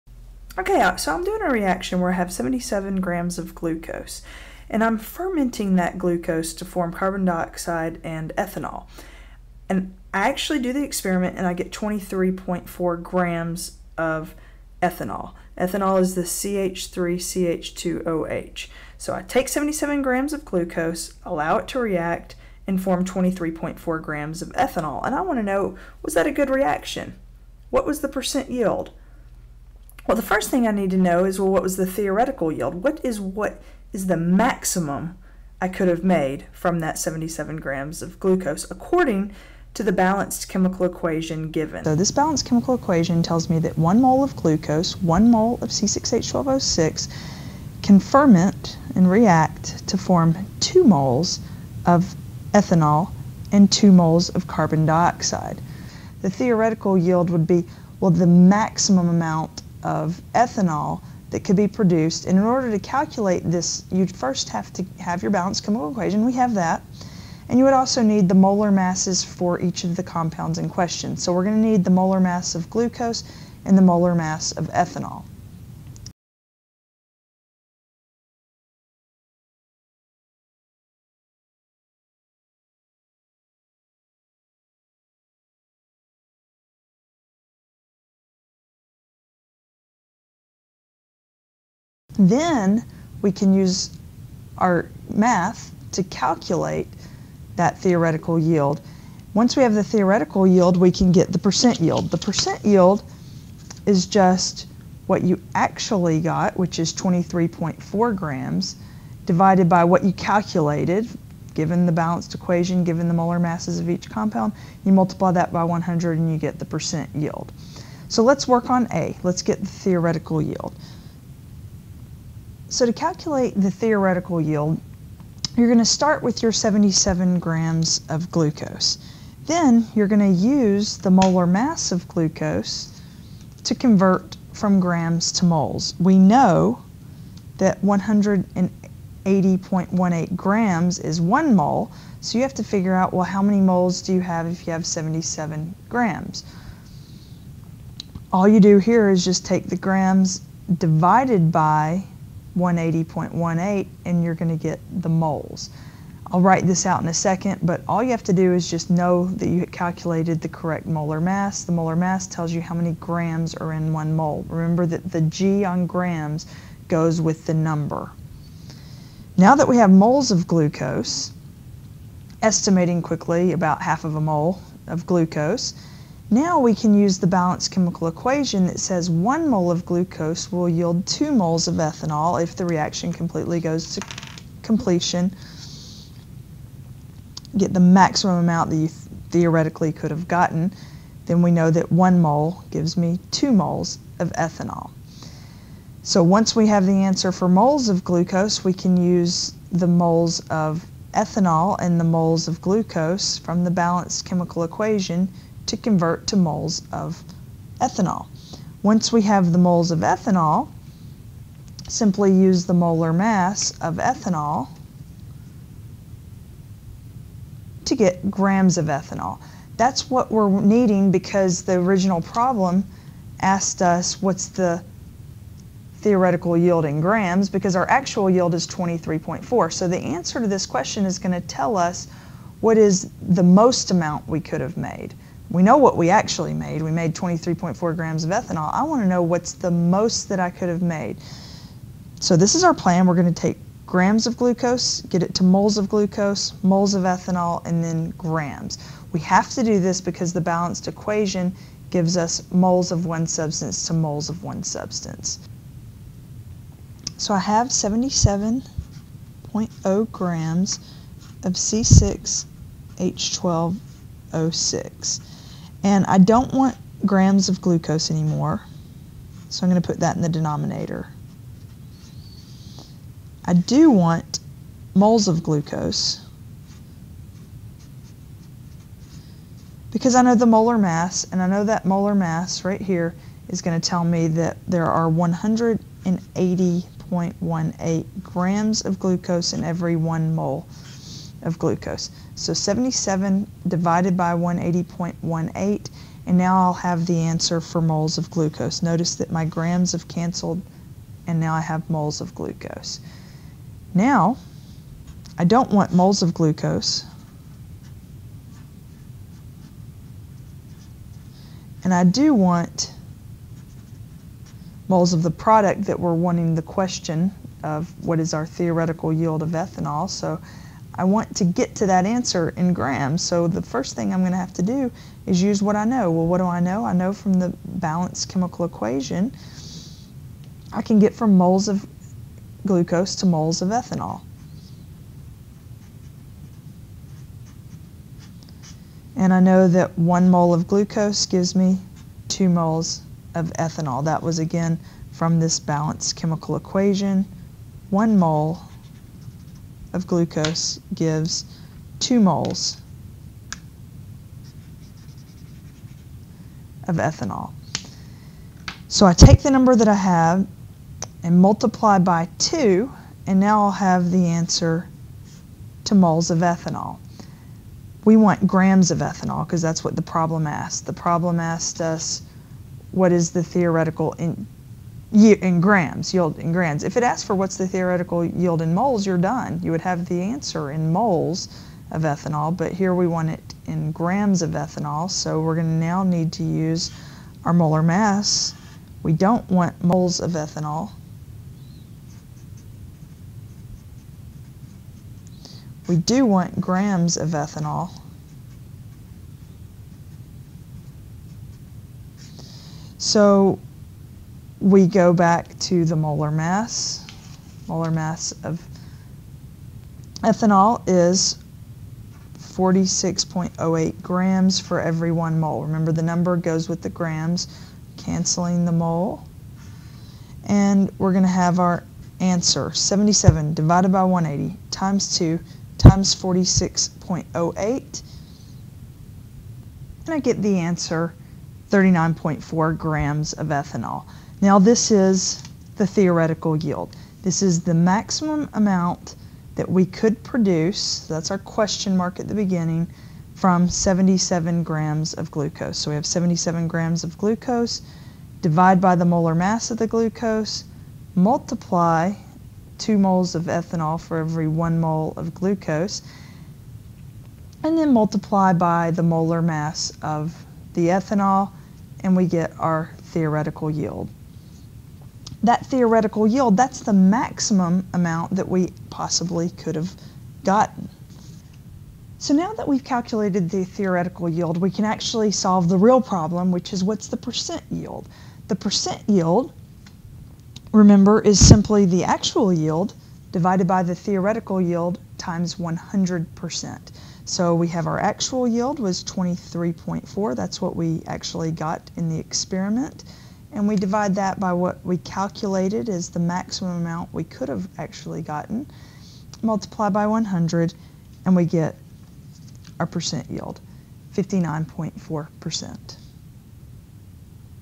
Calculating Theoretical Yield and Percent Yield in Chemistry
Worked Problem:   77.0 grams of glucose (C6H12O6)  is fermented to form 23.4 g of ethanol (CH3CH2OH).  Use these chemical formulas and the given balanced chemical equation (1 mole of glucose reacts to form 2 moles of ethanol and 2 moles of carbon dioxide) to calculate:

(a) the theoretical yield, and
(b) the percent yield for the reaction.

This video is intended as a general chemistry review.  I assume you remember and understand the concept of the mole and molar mass.  If not, here is a link that I found that my help you

I hope this helps!   Best wishes to you on your journey.